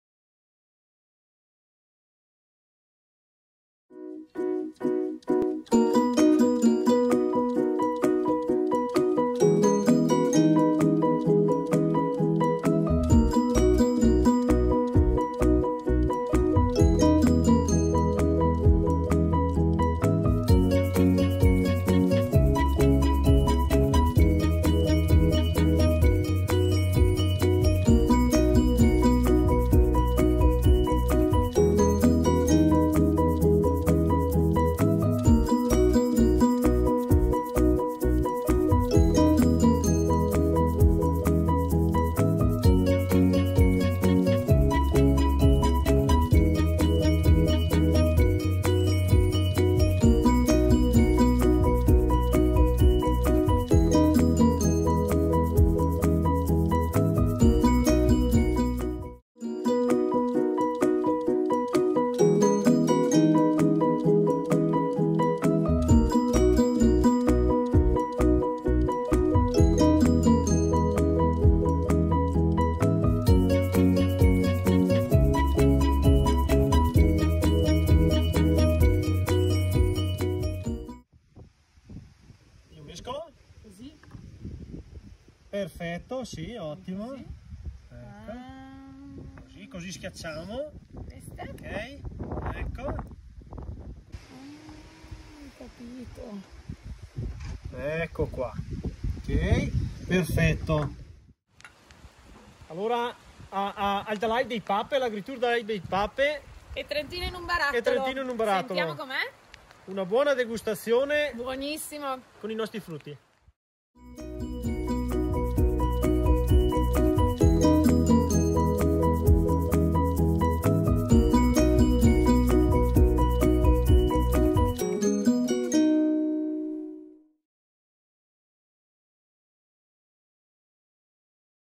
Trentino in un Barattolo all'Agritur Dalaip dei Pape | Sciroppo di sambuco e menta
Il gusto fresco dell’estate: lo sciroppo di sambuco e menta.
Per chi non avesse potuto partecipare all'appuntamento con TrentinoinunBarattolo all'Agritur Dalaip dei Pape, oggi vi sveliamo la loro ricetta per imparare come ricavare uno sciroppo dal gusto fresco e dissetante dai profumatissimi fiori di sambuco insieme alle foglie di menta piperita!